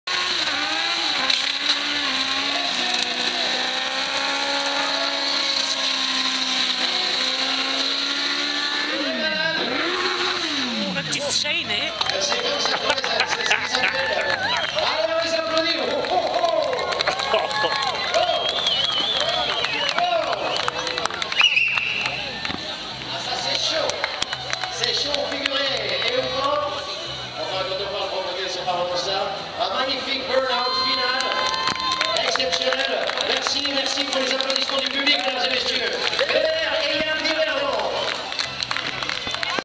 Burn out.MP4
Verbier Biker Fest - Stunt riders doing a burn out.